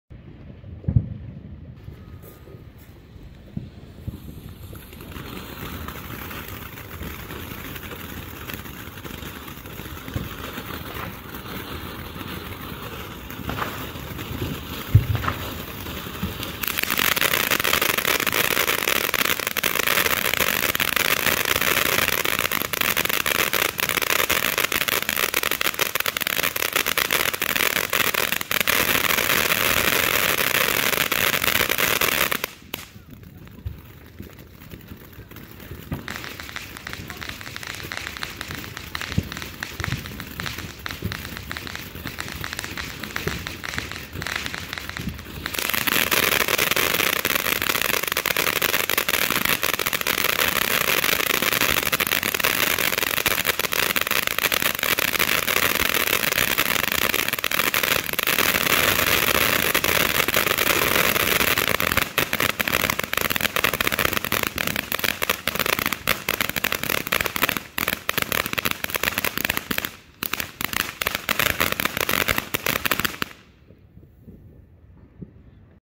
Glowing Lotus by Pagoda
Thanks to @redbeardsfireworksshowcase i was able to try a few of these out! Great fountain I'd definitely get again.